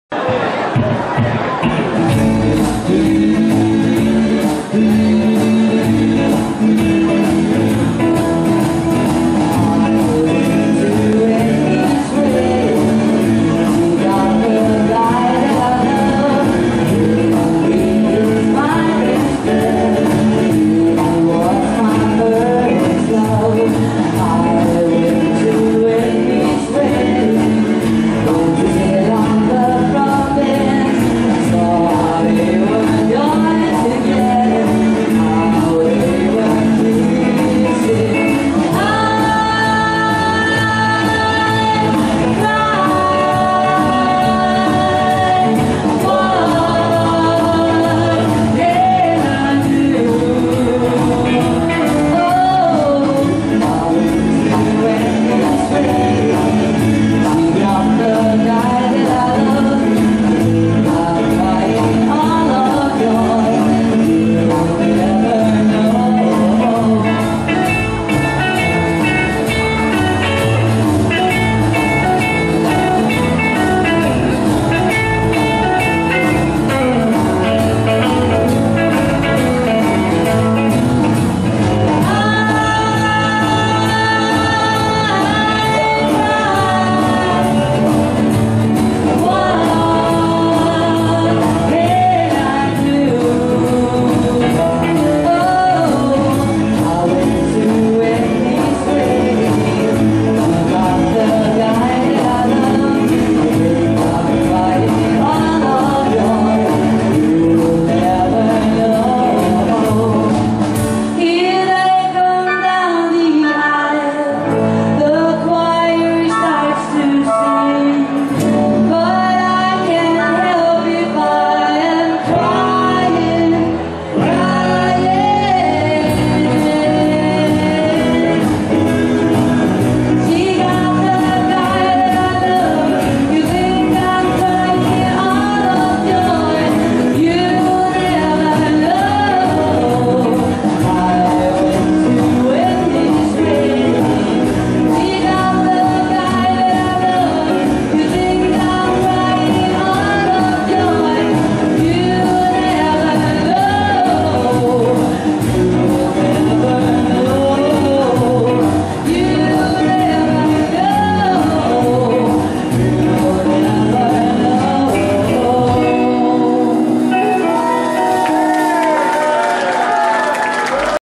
Eva Eastwood & Her Band HEMSBY 48
Eva Eastwood & her band @ Hemsby 48, filmed by uwe40seven